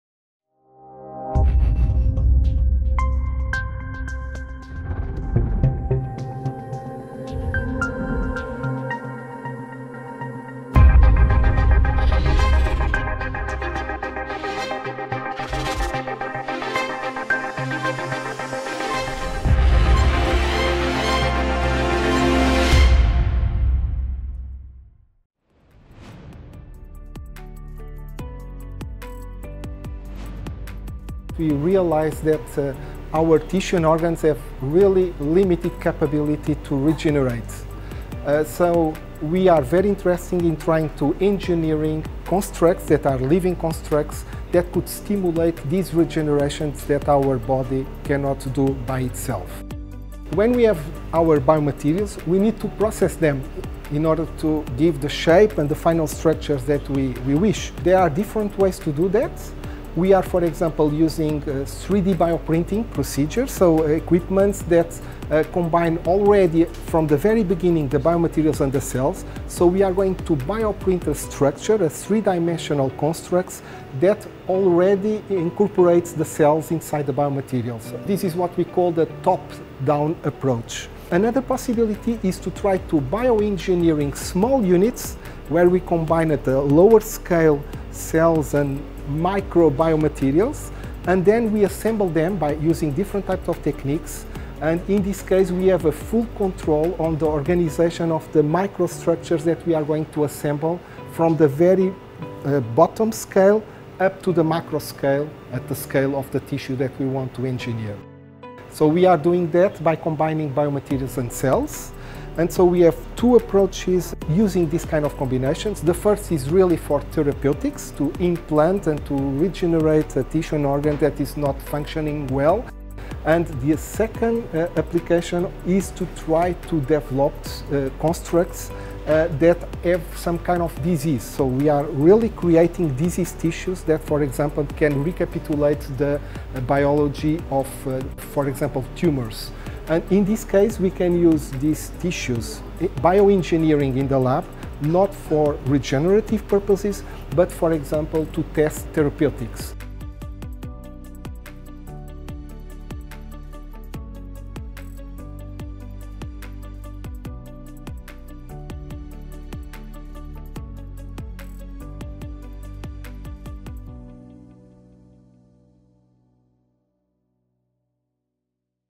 Ep.11 | João Mano - From Biomimicry to Breakthroughs: The Rise of Bio-Inspired Materials
João Mano, a full professor form CICECO, and his team are engineering living constructs to stimulate tissue regeneration where the body cannot heal on its own. Various methods exist to shape and build these living materials, such as 3D bioprinting and bioengineering small units like micro biomaterials. These techniques are applied for therapeutic purposes, including implanting and regenerating malfunctioning organs or creating constructs that mimic the biology of diseases (like tumors) to test potential treatments.